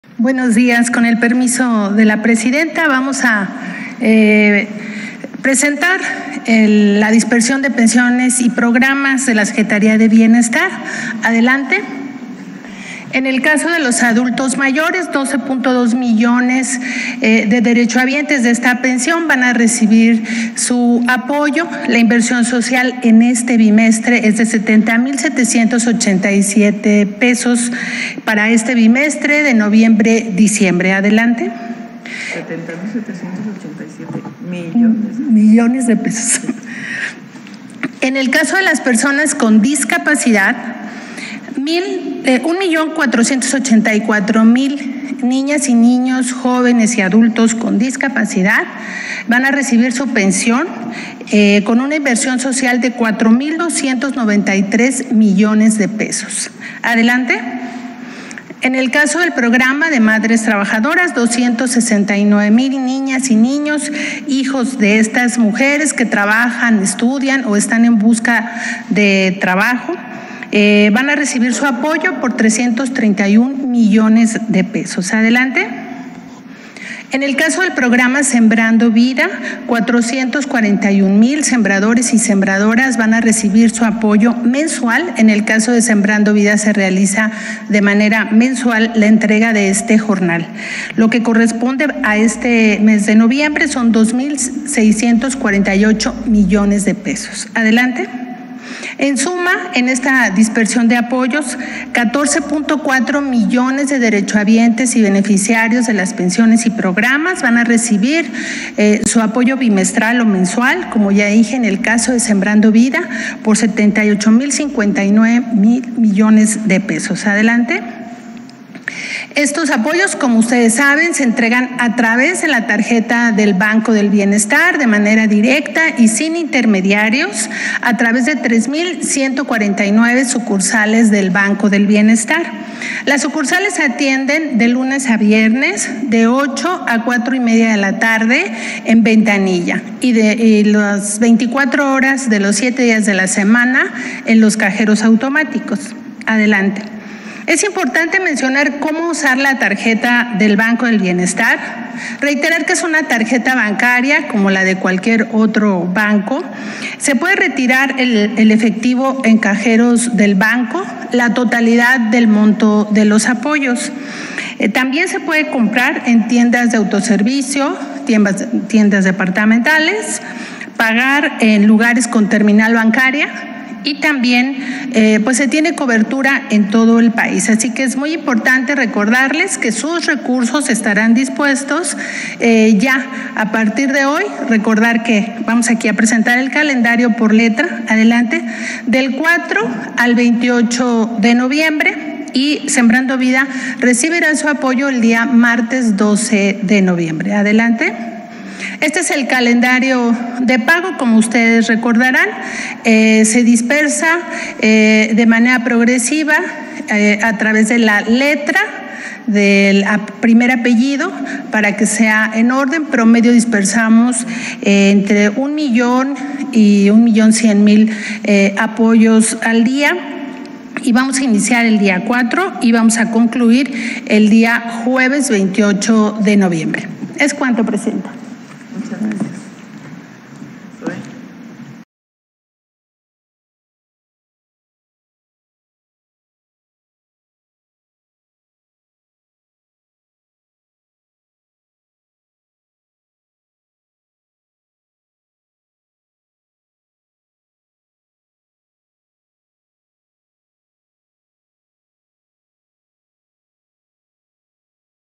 ¡CALENDARIO CONFIRMADO! - Estas son las fechas del pago de las pensiones Bienestar - Noviembre 2024
DOBLE PAGO Beca Benito Juárez Noviembre 2024 ¿Cómo Recibirlo?

Mejora tu OFICINA
📷Mi Cámara
🎤Mi Micrófono
💻Mi Laptop
🖱️Mi Mosue
🖥️Mi Escritorio
🪑Mi Silla
Mi Teclado